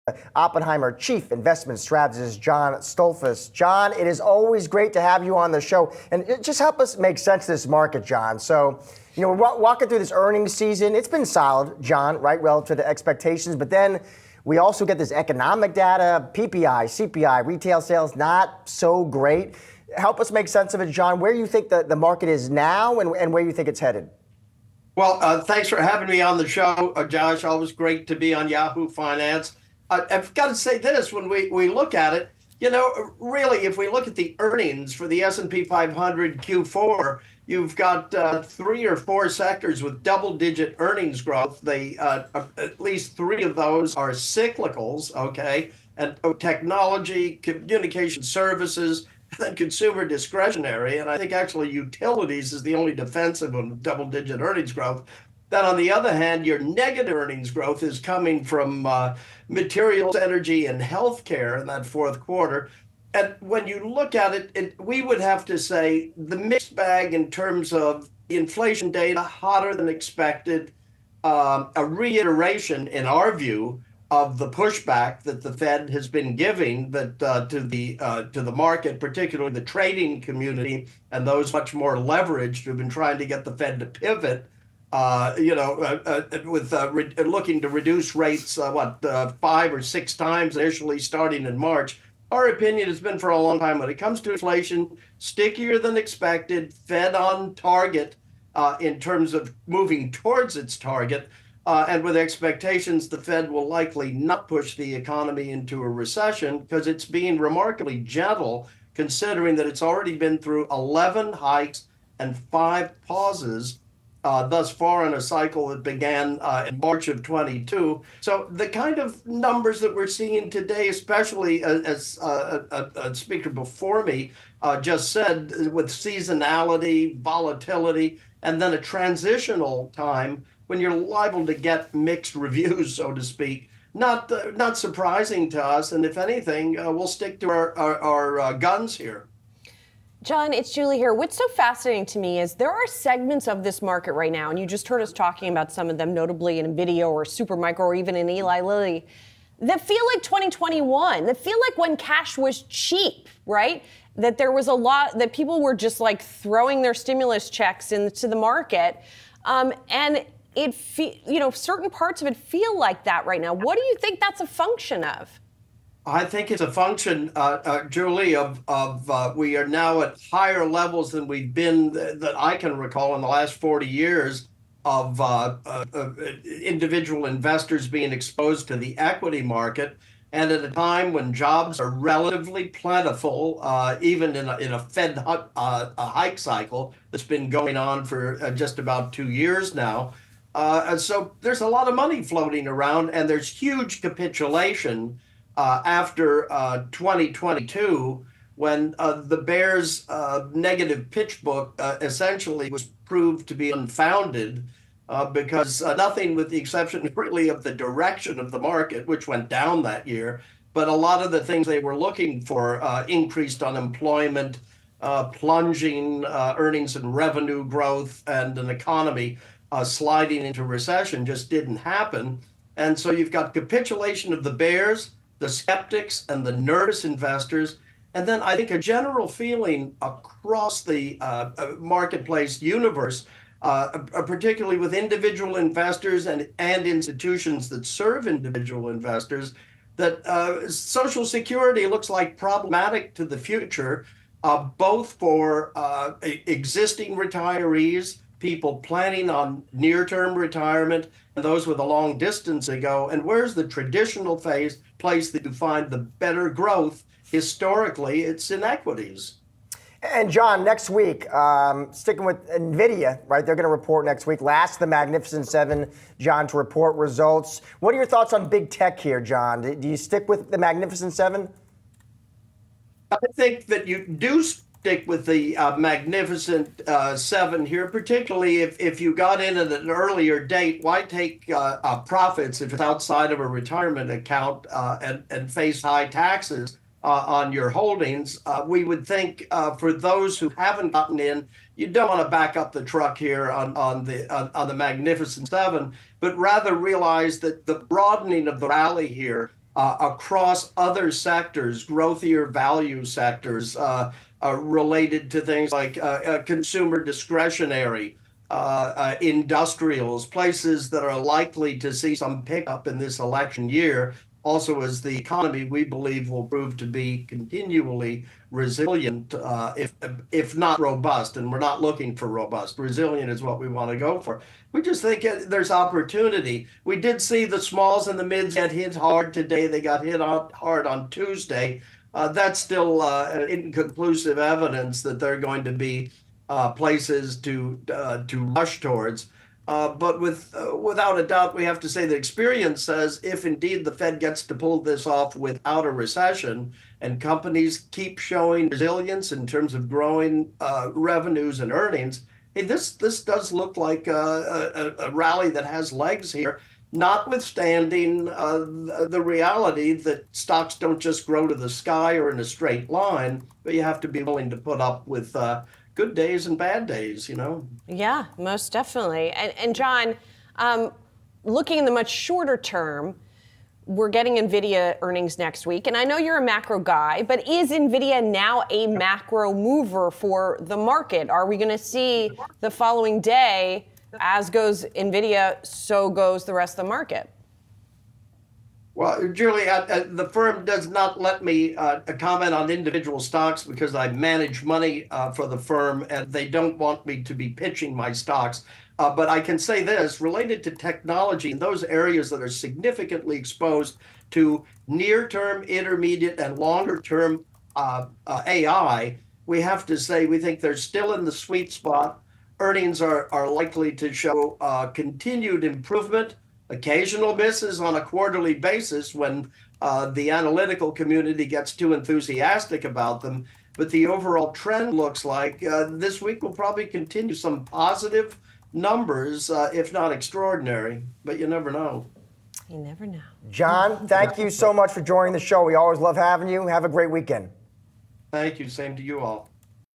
Market volatility product of hot data, 'gentle' Fed policy
Oppenheimer Chief Investment Strategist John Stoltzfus joins Yahoo Finance to analyze market reactions following numerous hot inflation prints this week.
Stoltzfus says the volatility reflects "pushback" against premature interest rate cut hopes after the Federal Reserve's hawkish messaging. He notes the Fed has been "remarkably gentle" in lifting rates, aiming to cool inflation without sparking a recession. Given this gradual approach of eleven hikes and five pauses, Stoltzfus says mixed market reactions are "not surprising."
Despite the turbulence, Stoltzfus points out there is substantial money still invested in equities from both institutional and individual investors. Markets stay stimulated with "a lot of money floating around," Stoltzfus explains.